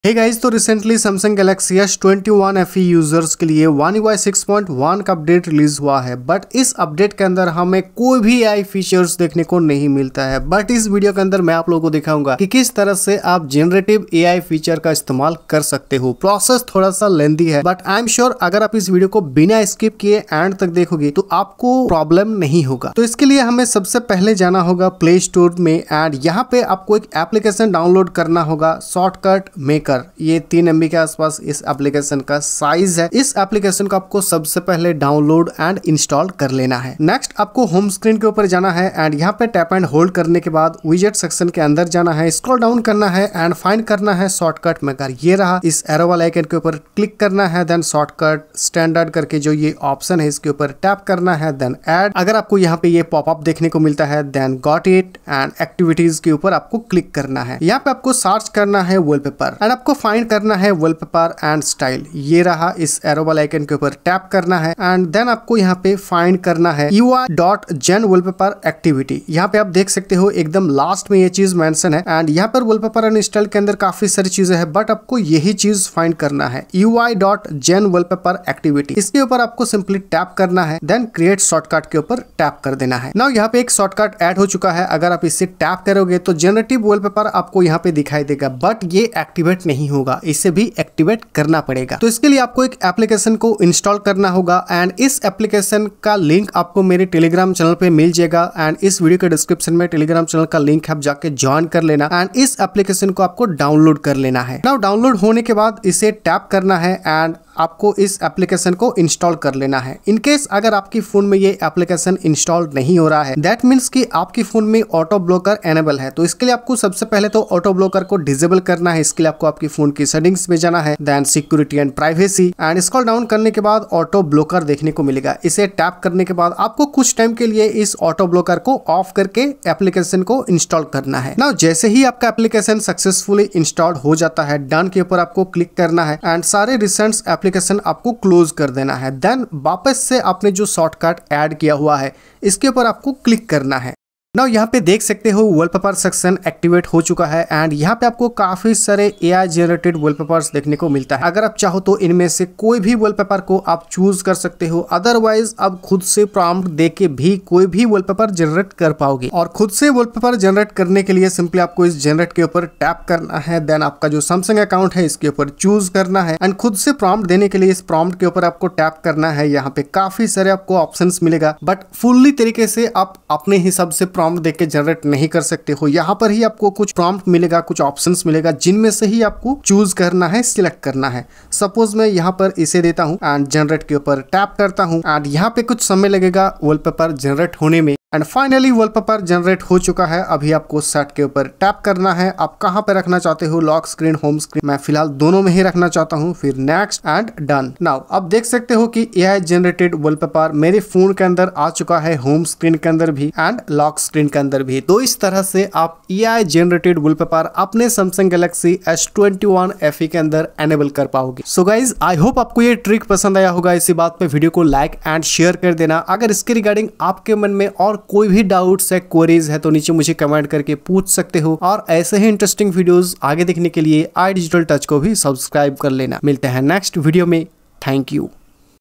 Activate Generative Wallpapers on S21 FE One Ui 6.1
Samsung introduced Galaxy AI with the One UI 6.1 update. Many Samsung Galaxy devices got Galaxy AI features with this update, but Samsung did not provide any Galaxy AI features on the Samsung Galaxy S21 FE 5G. In this video, I will share a trick on how to enable generative AI wallpaper features on the S21 FE 5G. So if you are an S21 FE user, watch this video!

🎙️ Lap Mic
🎙️ Voice Over Mic
💡 Key Light
💡 Ring Light
💡 RGB Light